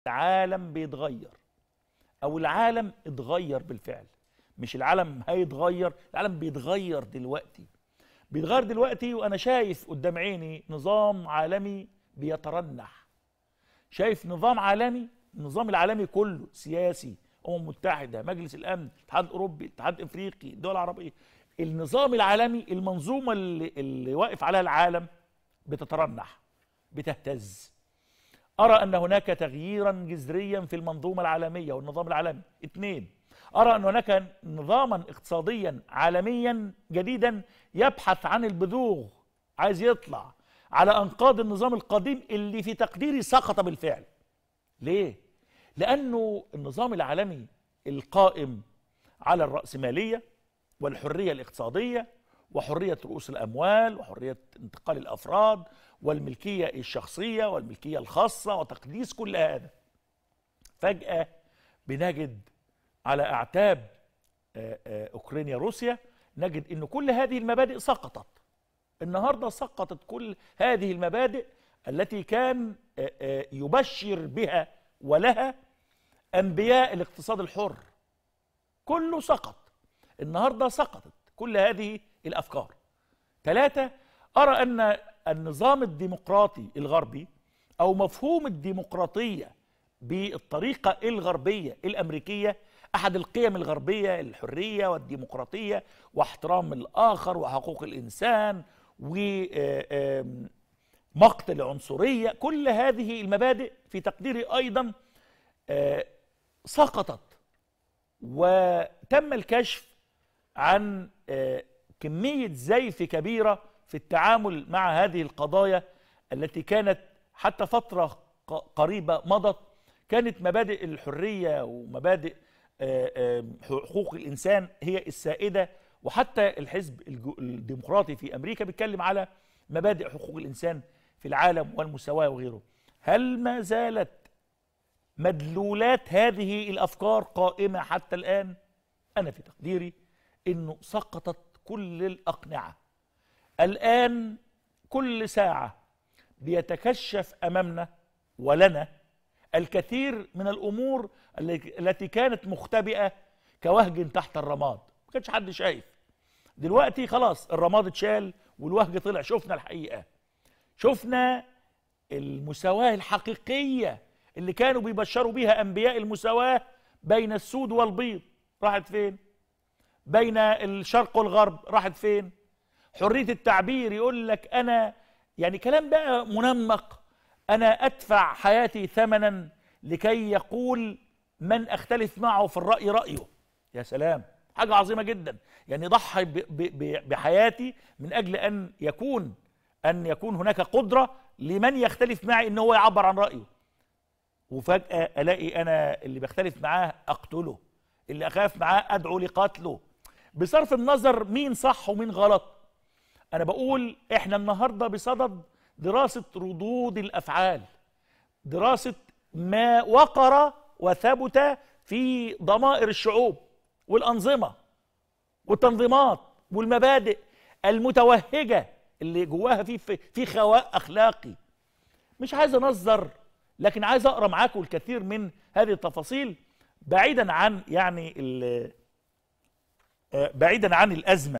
الديهي: النظام العالمي يترنح ويهتز وهناك تغيير جذري واقتصاد عالمي جديد والنظام الحالي سقط بالفعل
• تابعوا الصفحة الرسمية للإعلامي نشأت الديهي على الفيسبوك ➭